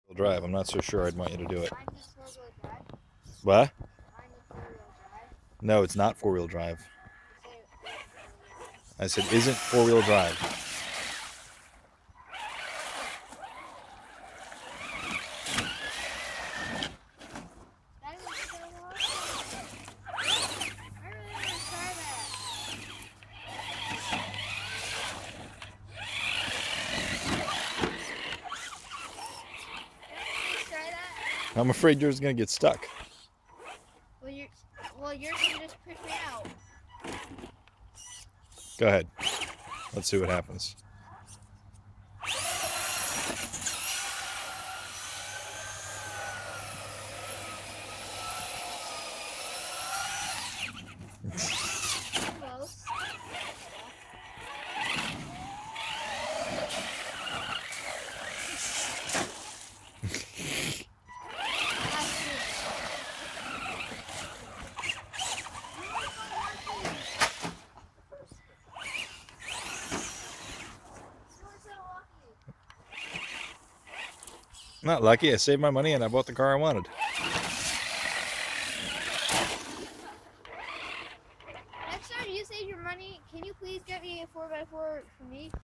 RC trucks on the pool cover
Driving RC trucks on the pool cover.
A 4 wheel drive ECX Torment, and a 2 Wheel drive Traxxas Raptor.

I was driving the Torment with one hand while trying to hold the camera, so the driving is pretty sloppy.